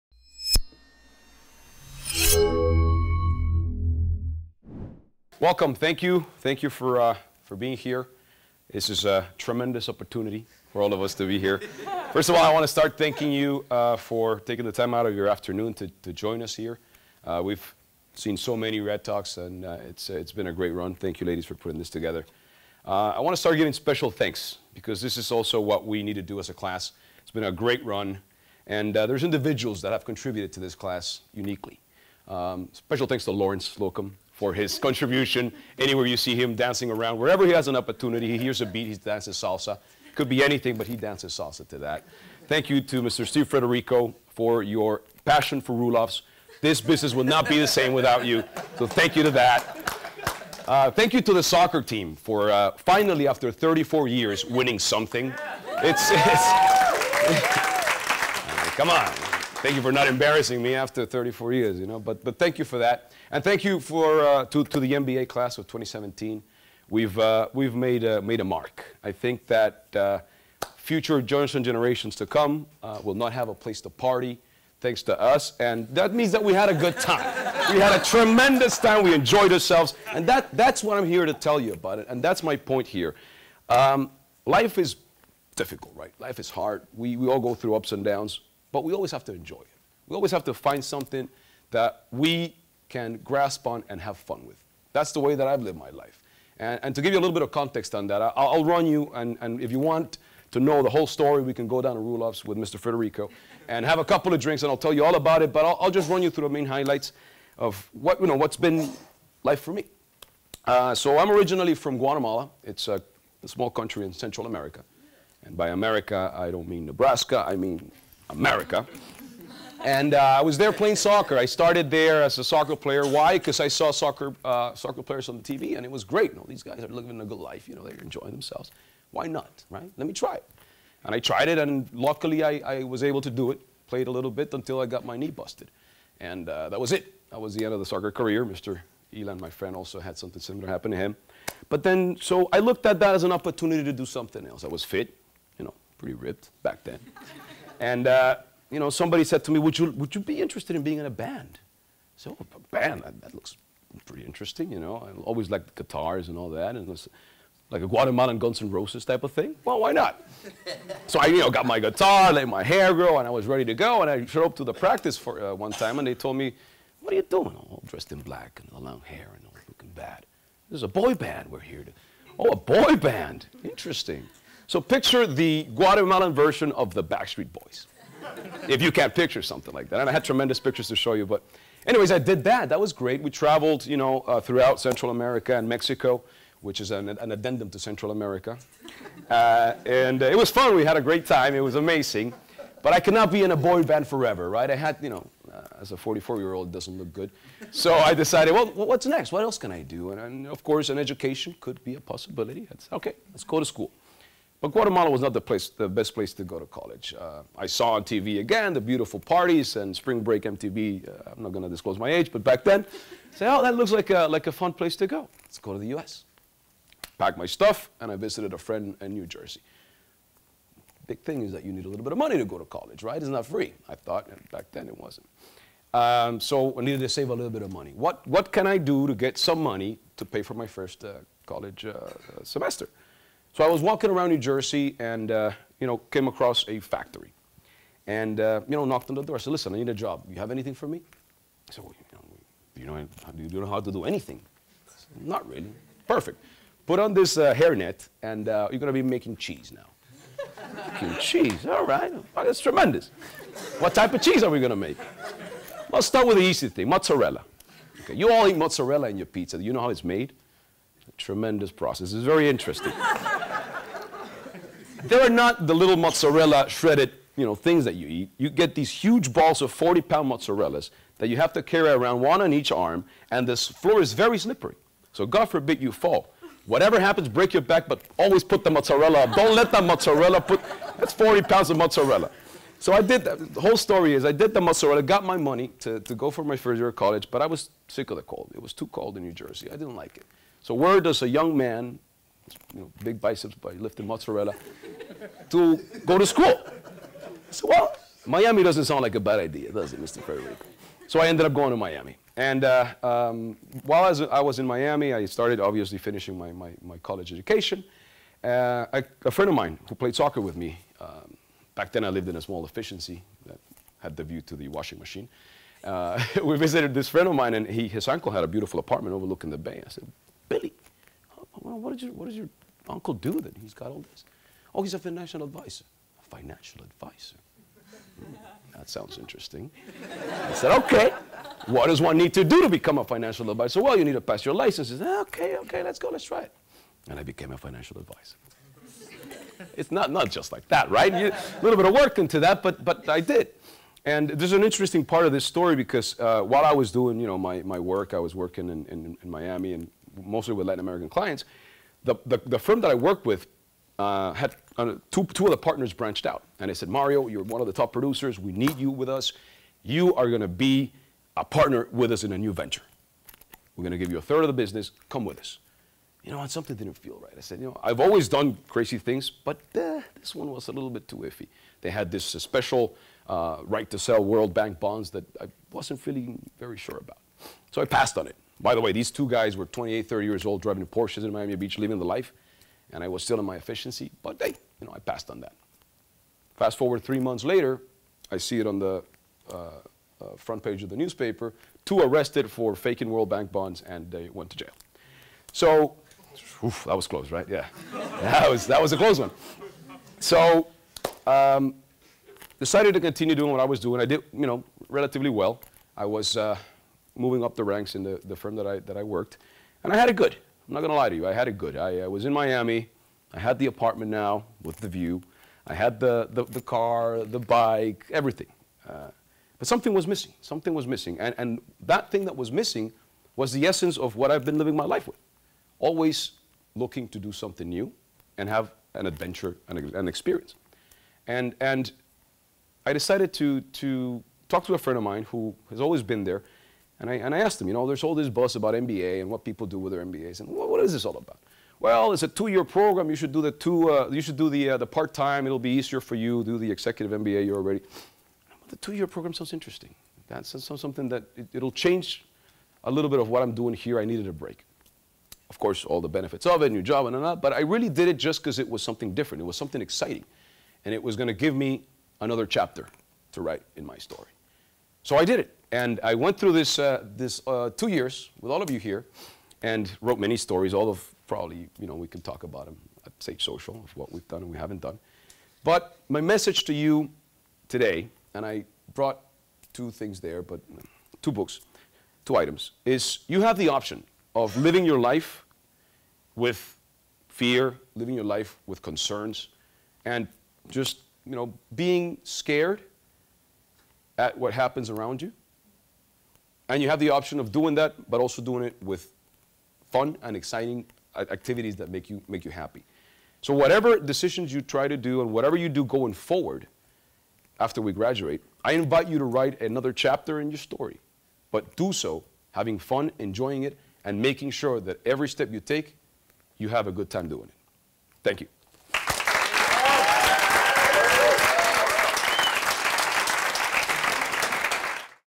REDtalks: Mario Saravia, MBA ’17
Mario Saravia, MBA ’17, delivered one of 2017's final REDtalks as part of Johnson's year-end GRADtalks series. An ex–soccer player, boyband member, and mozzarella-maker, Mario talks about the importance of having fun when life gets difficult.